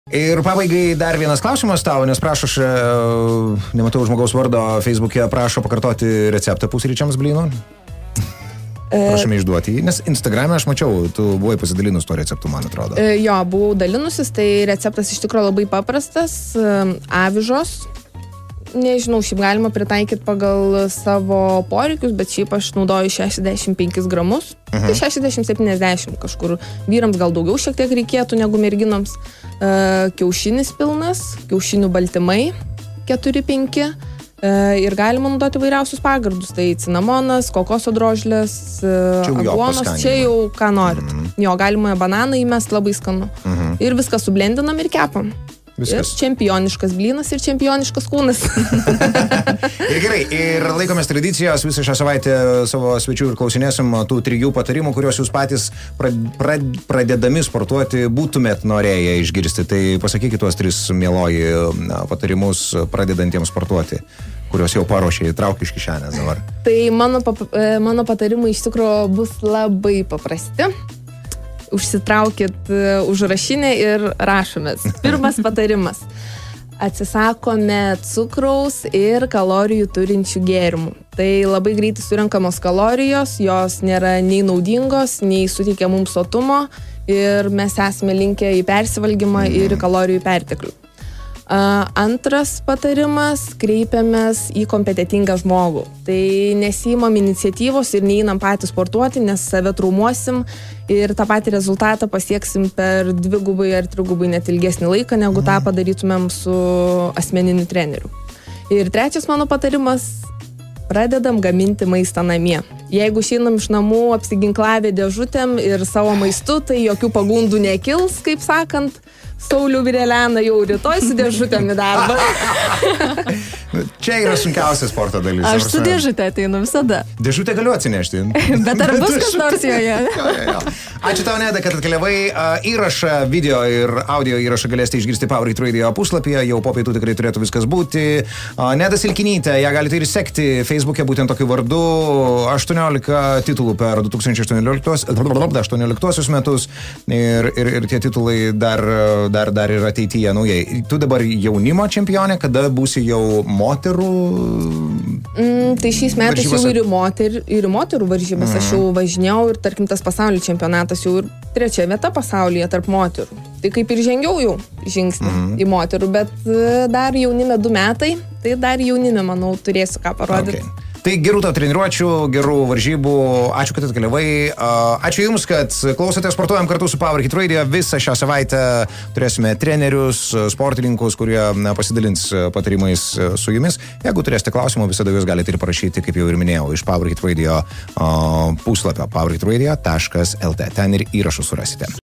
Nedos čempioniškų blynų receptas
Pasaulio bikini čempionė rekomenduoja tau pabandyti šiuos blynus! Puikus patiekalas sportuojančiam!

Hey, tau reikės:
65 - 70 g. avižų
1 pilno kiaušinio
4-5 kiaušinių baltymų
Pagardų - bananai, uogos ir pan.
Viską sublenderiuojam ir kepam!!!

Pažiūrėk ir šitą "Vėl tie patys - Michael Jordan ir Kobe Bryant - kas geresnis?"